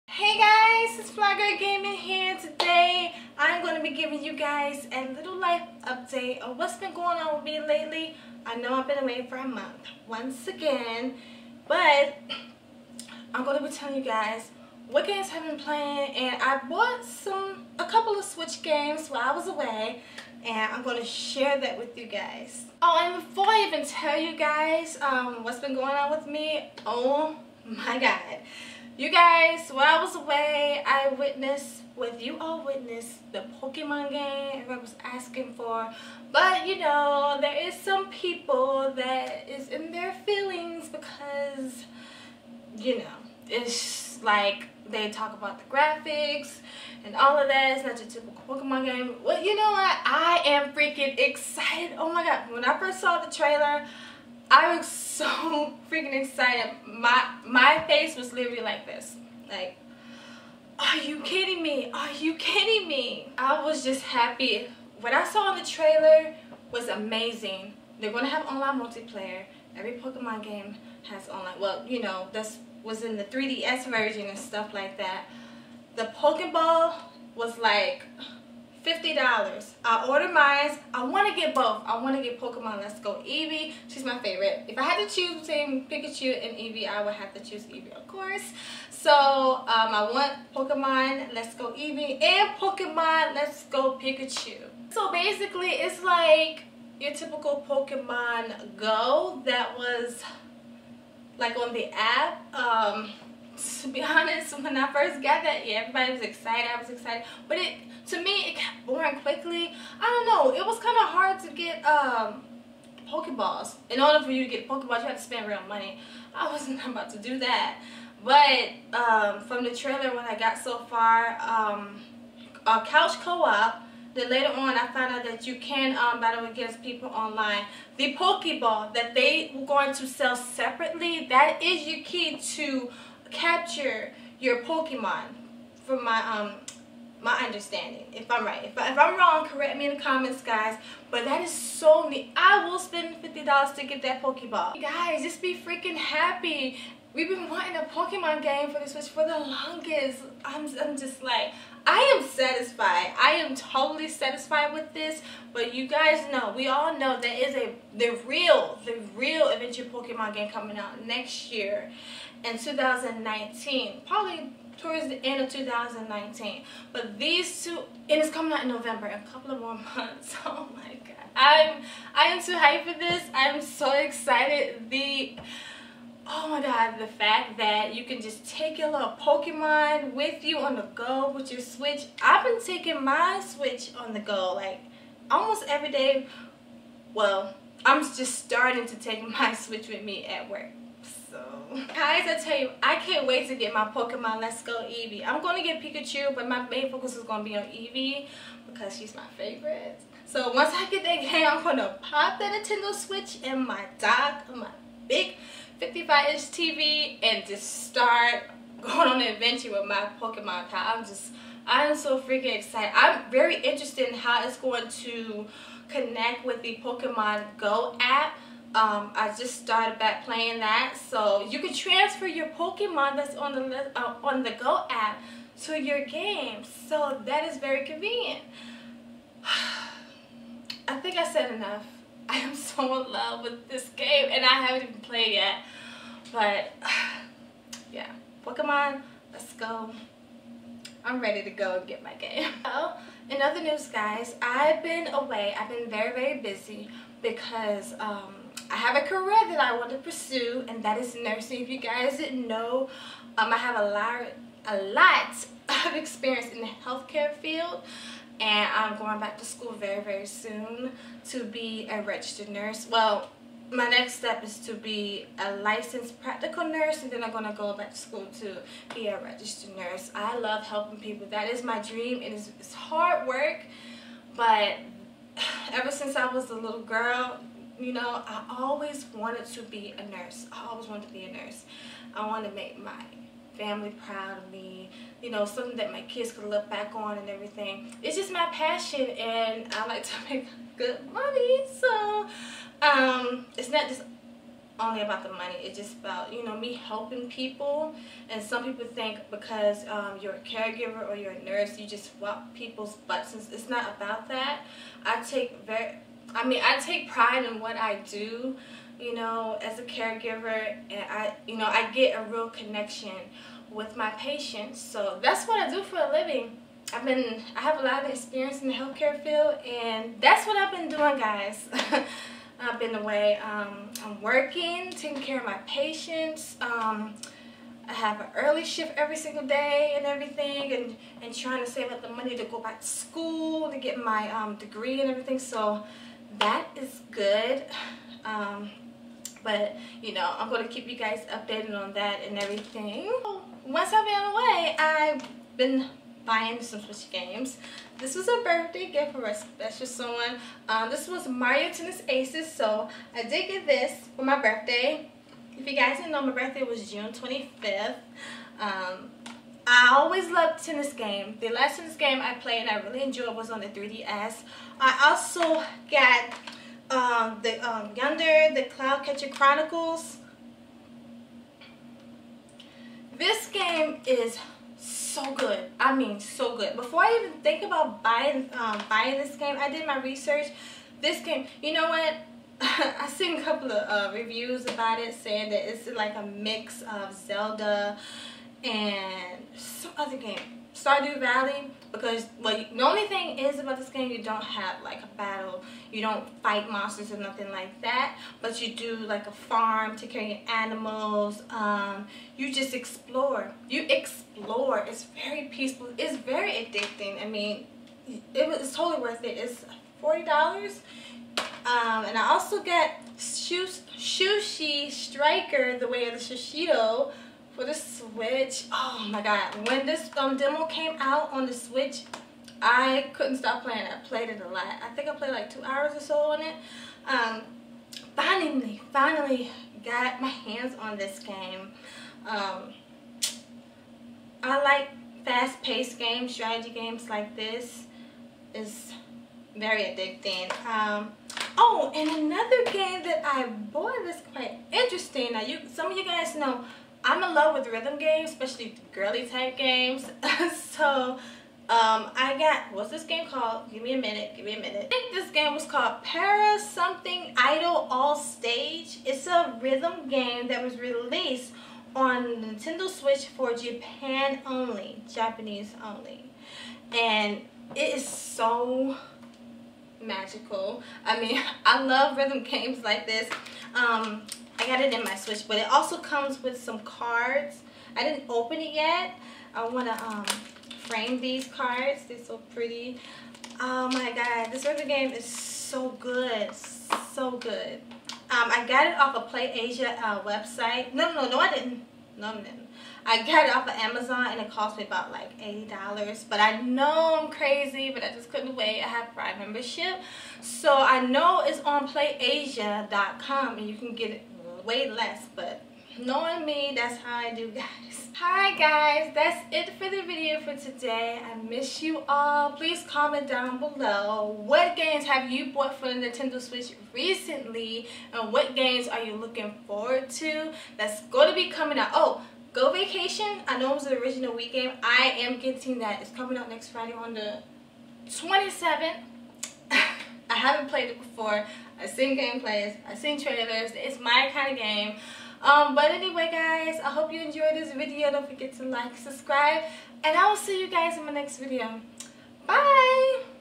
Life Update+OMG Pokemon Lets Go! More Switch Games
Hey Guys!
I'm back once again i been super busy with life so much has happen while i was away. Here is a little update on me and a few games i picked up